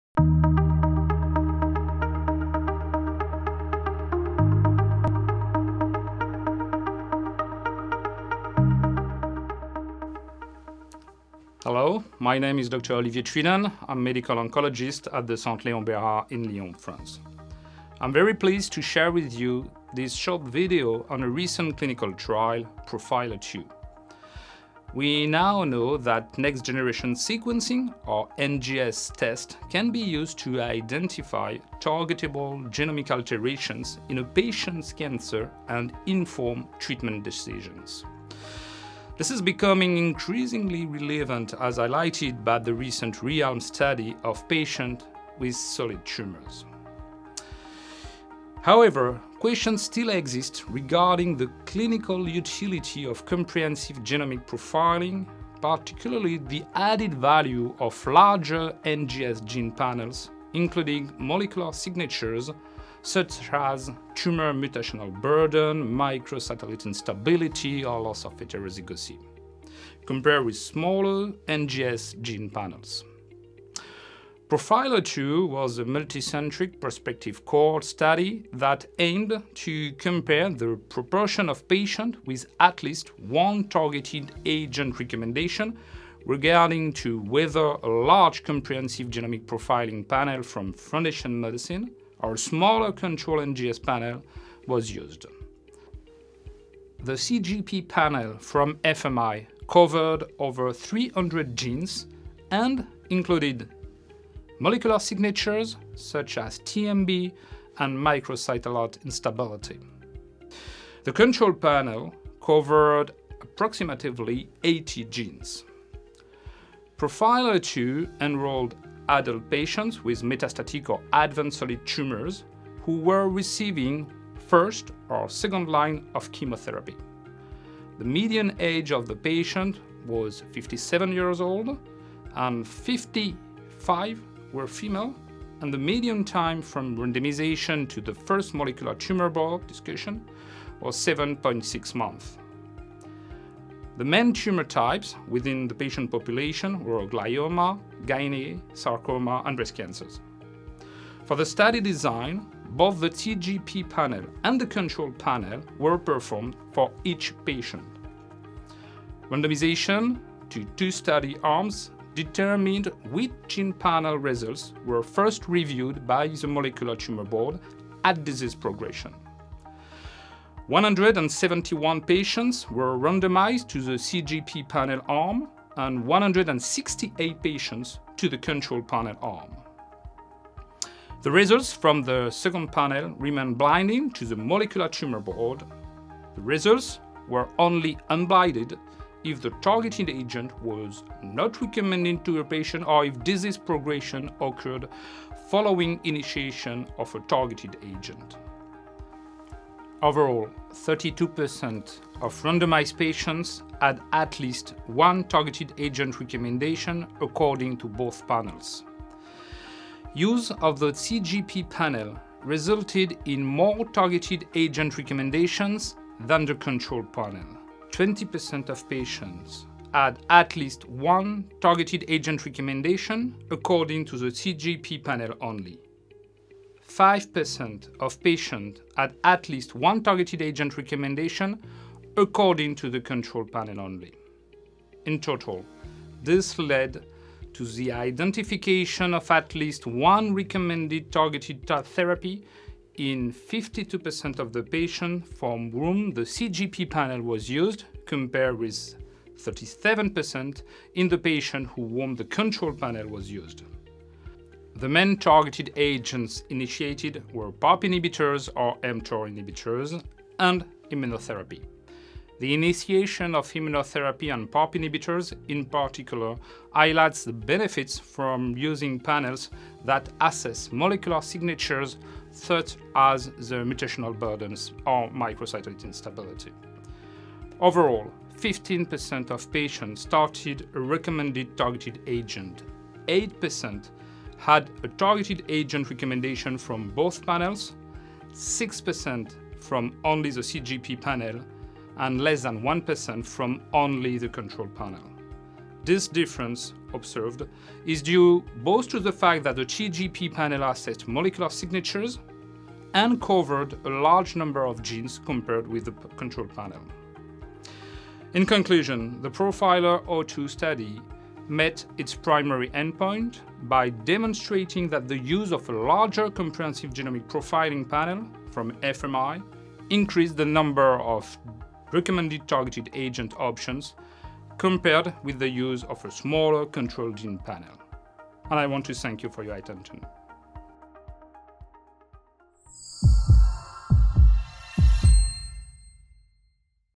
Key findings from the PROFILER 02 study
Next-generation sequencing (NGS) gene panels can be used to identify targetable genomic alterations in a patient’s cancer and inform treatment decisions. However, is there an added value to using comprehensive genomic profiling which utilises larger NGS gene panels with molecular signatures, compared with smaller NGS gene panels?

To learn more, watch this video where Dr Olivier Trédan explains the key findings from the recent investigator-initiated study (IIS) PROFILER 02 (NCT03163732).